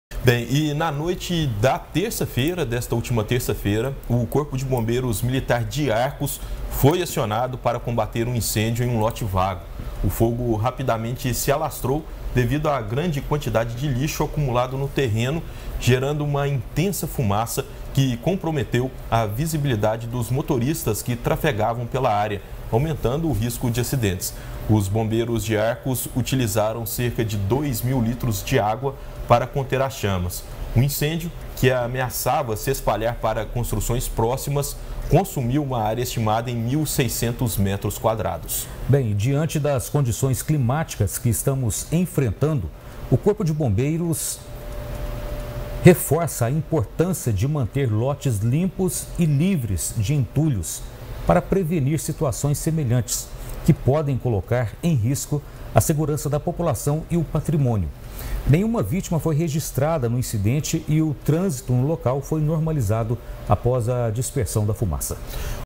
INCÊNDIO EM LOTE VAGO MOBILIZA CORPO DE BOMBEIROS EM ARCOS
| REGIÃO |

Na noite desta terça-feira, o corpo de bombeiros militar de arcos foi acionado para combater um incêndio em um lote vago. O fogo rapidamente se alastrou devido à grande quantidade de lixo acumulado no terreno. Saiba mais: